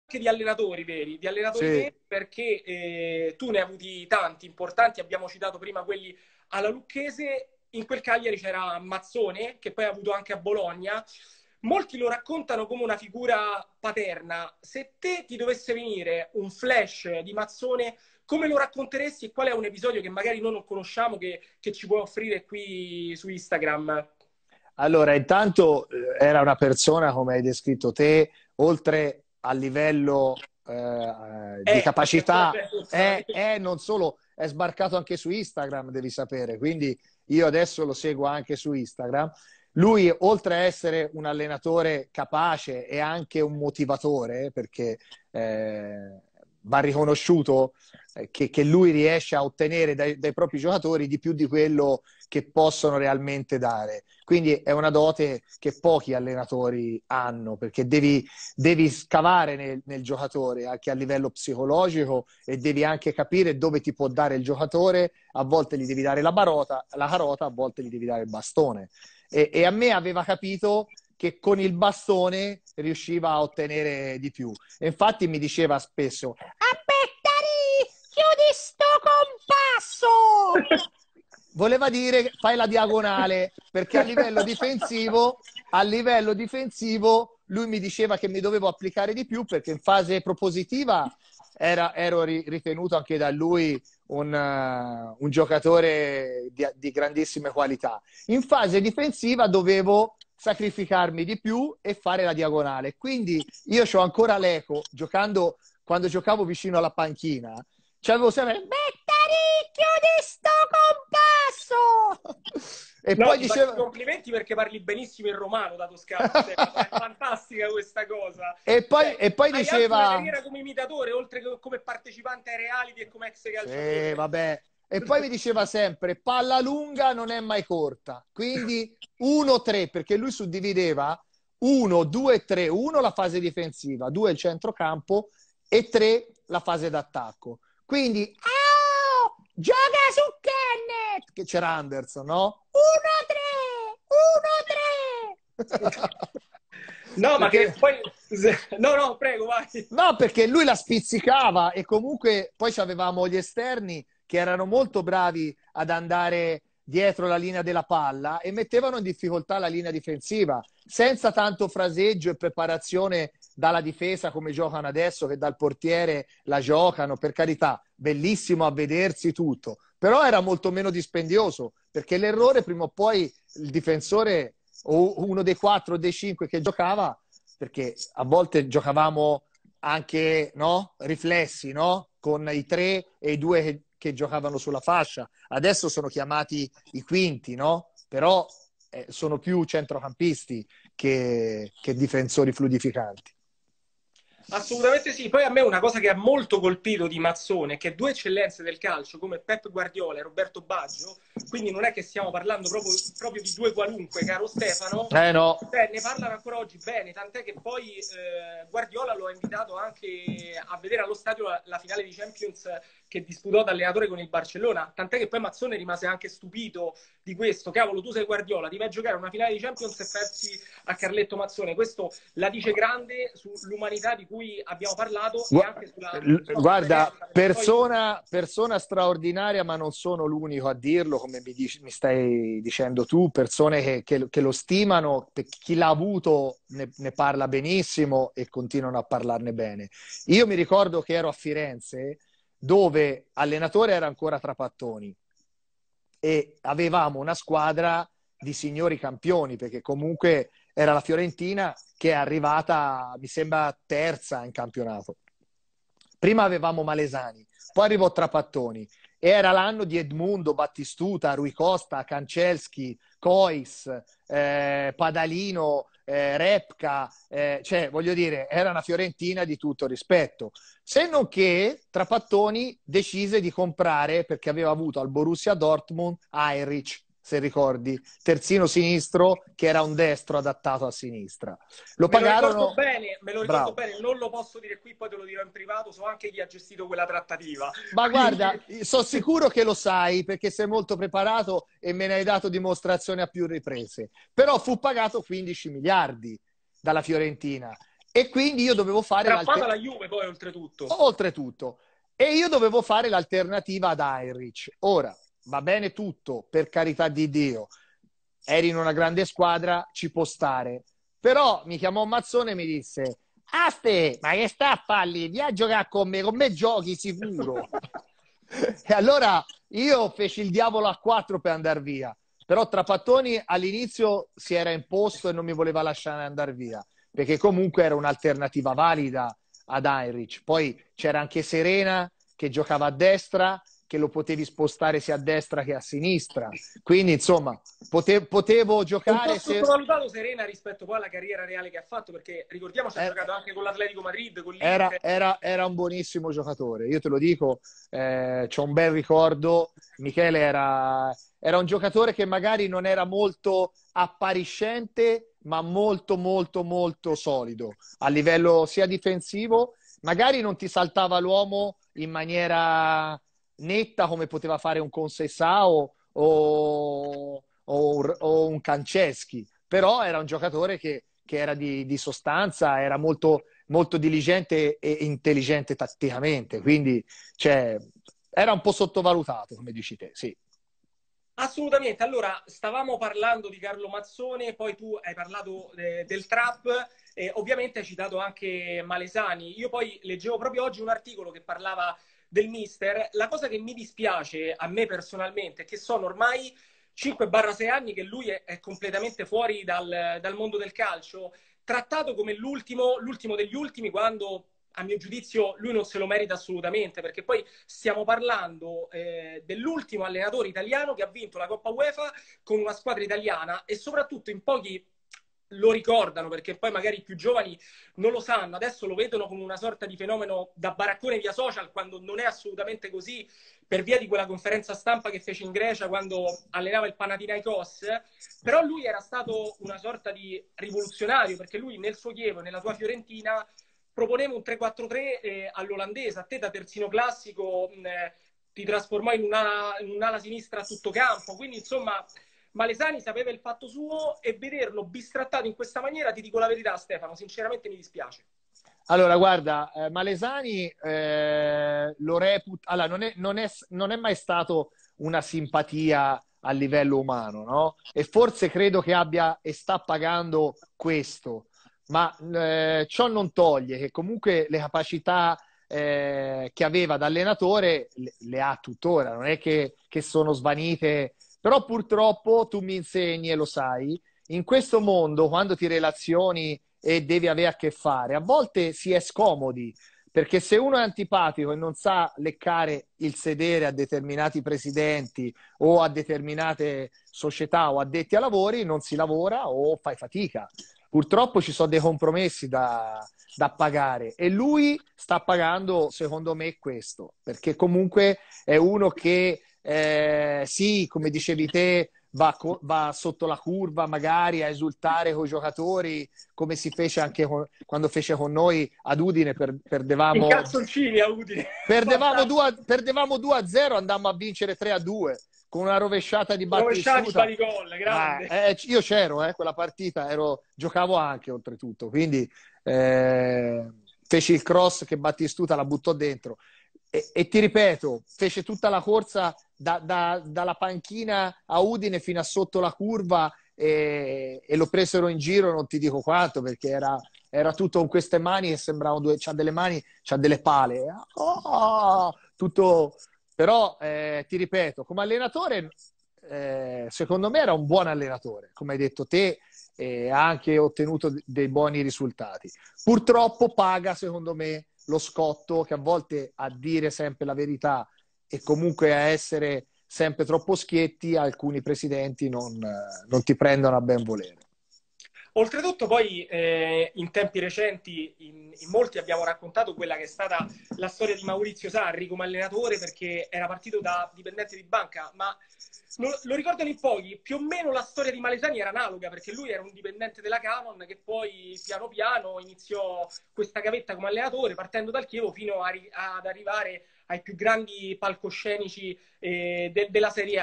Stefano Bettarini su Carlo Mazzone e Alberto Malesani
Stefano Bettarini parla a 360° con Francesco Vitale dei periodi in cui da giocatore è stato allenato da Carletto Mazzone e Alberto Malesani.

Diretta Instagram del 25/02/2021